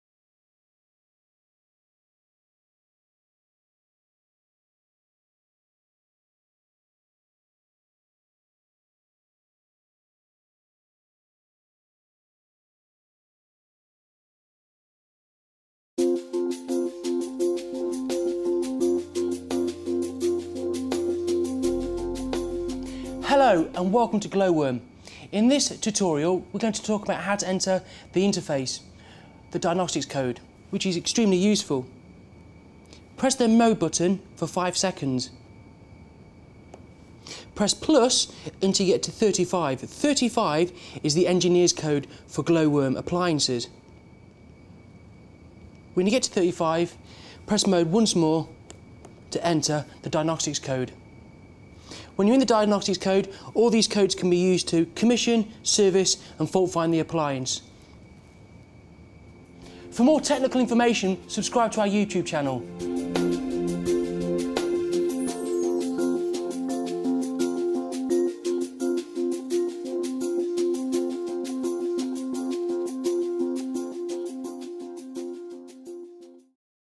Diagnostics - Accessing the diagnostics codes on Glow-worm boilers
An engineer’s guide to accessing the diagnostics codes on the Glow-worm Energy, Betacom3,Procombi Essential, Easicom2 and Ultimate2 boilers .

(Not applicable for the  Energy 35 Store)